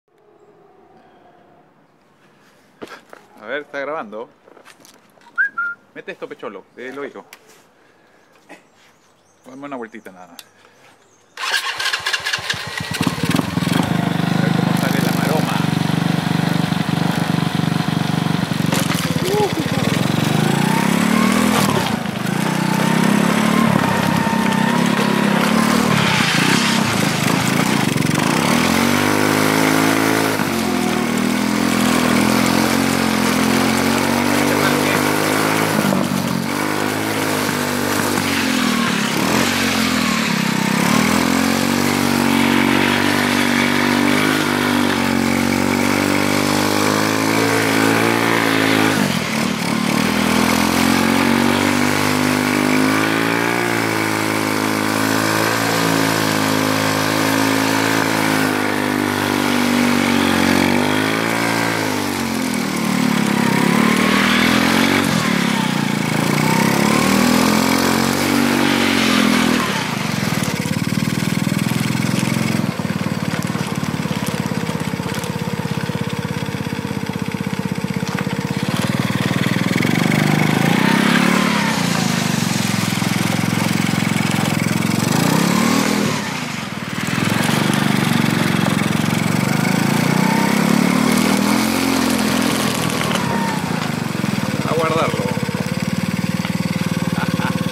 TRIBIKE TRESPA
Aqui probando el tribike con motor y llantas de kart